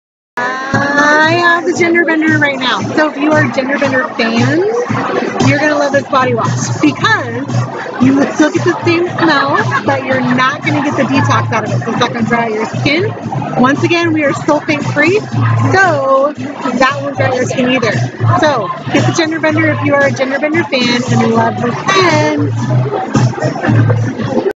Seattle Day Away Product Sampling and Review of the Gender Bender
Join Renee Anderson with Perfectly Posh at Seattle Day Away as she reviews some new Spring Line  Products.

The Gender Bender wash is a great musky smelling wash that is sulfate free. Great for men and Woman.

Join the fun at poshpamperingmom.po.sh/join